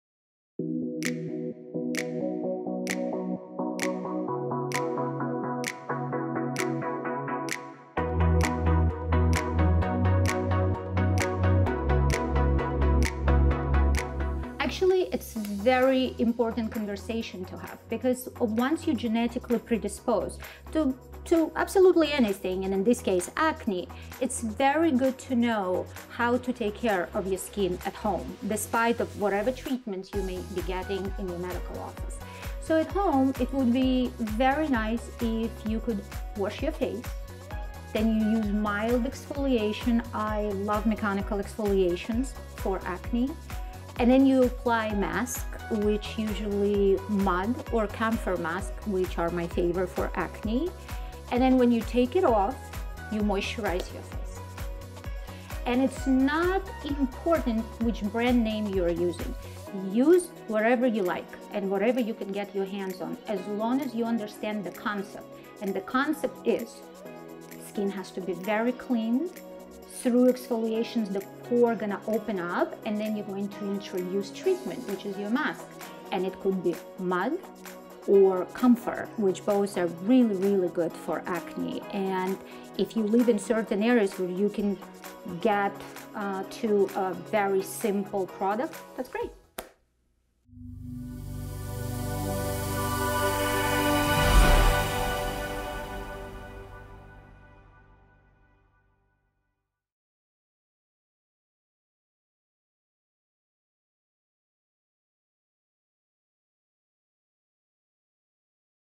Medical Aesthetics Education Series by Dr. Y - What is the best way to treat acne?
Hey Clinique YFT family! Today we’re going to address some facts about Acne that you might not know…

Dr. Y wants everyone to understand that some people are genetically predisposed to have worse acne than others. Still, it is something that everyone deals with, especially during our teenage years when hormonal influence is present.

As your hormonal production changes, the body has to adjust and rebalance how to manage the increase in estrogen, progesterone, or testosterone! Some of this rebalancing we can’t even see… but we don’t need a microscope 🔬 to see how it affects our skin.

Video Featuring: Dr. Yelena Yeretsky

CONNECT WITH DR. Y

LIKE THIS VIDEO? WANNA SAY THANKS?